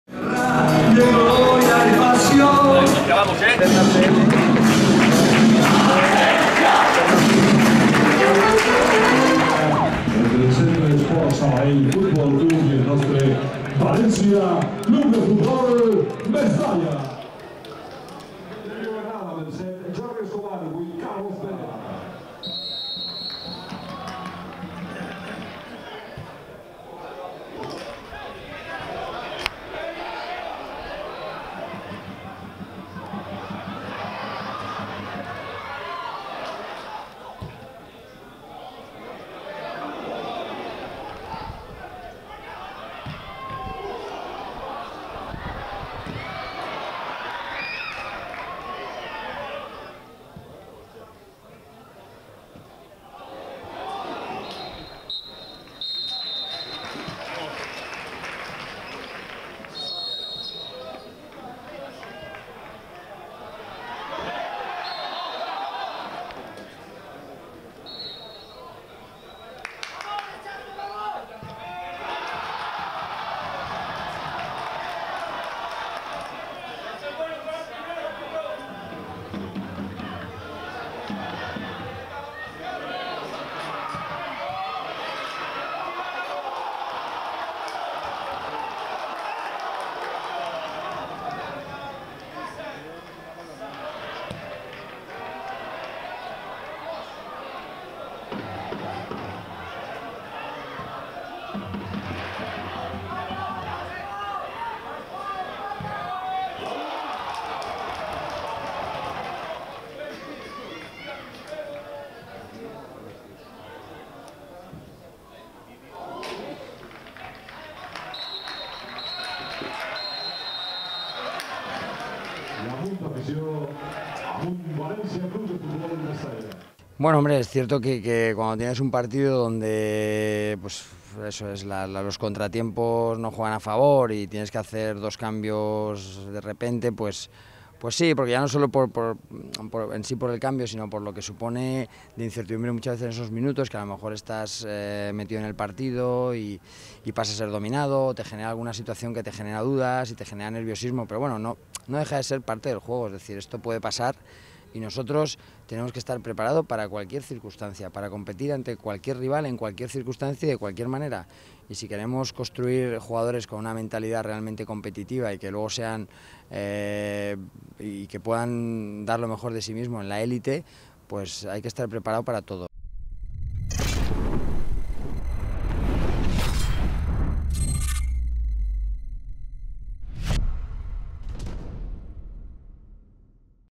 EL VCF MESTALLA PIERDE ANTE EL LÍDER CE SABADELL EN EL ANTONIO PUCHADES (0-3)
El equipo catalán sentenció el choque en la segunda mitad con dos goles en cuatro minutos.

The opposition clinched the game in the second half scoring two goals in 4 minutes.